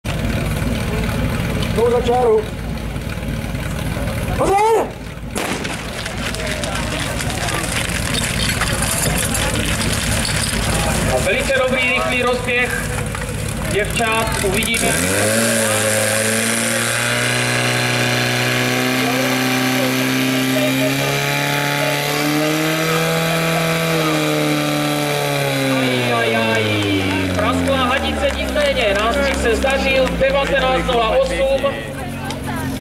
17.10.2015 SDH Hodějice ženy - Pohár Republiky Vacenovice
šport hadice - 2B (B65-C42)
sklopné terče